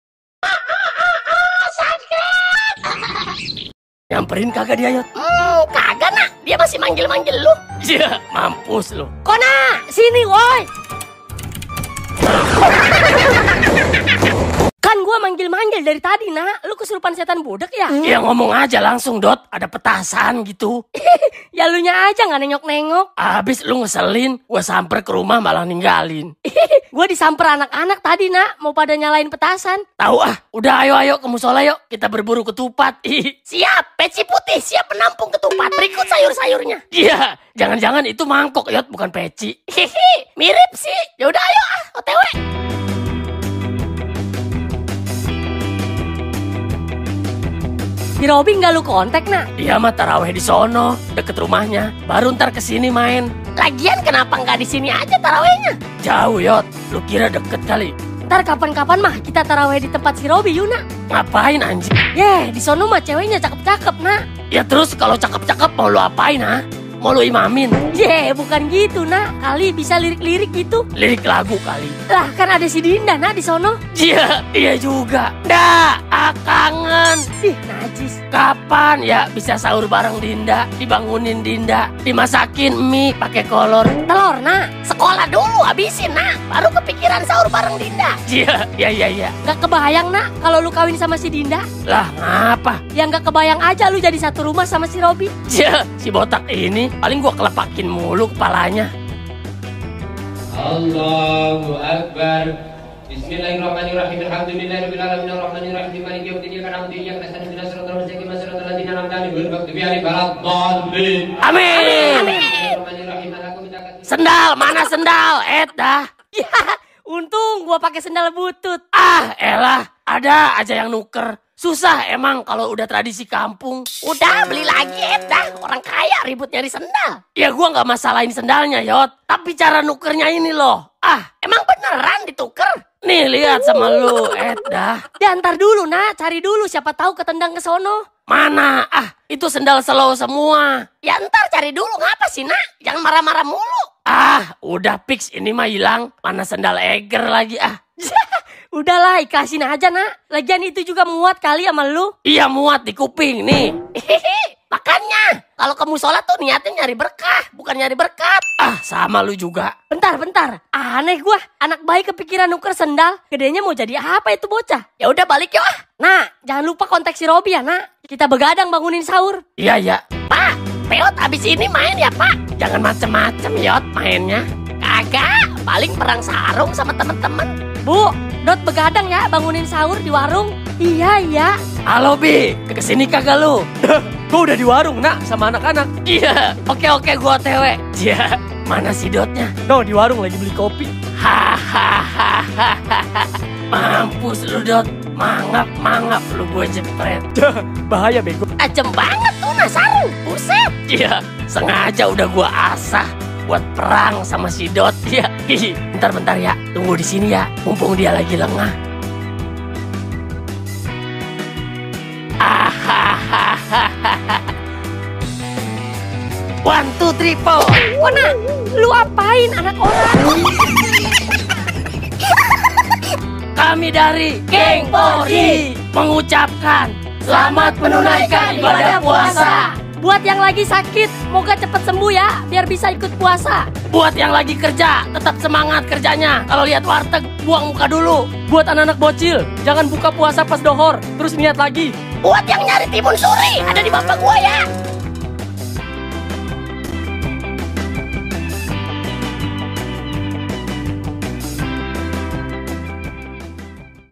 Geng 4G Edisi Ramadhan || kona perang sarung salah sasaran | Dhot Design
InsyaAllah berkah 🤲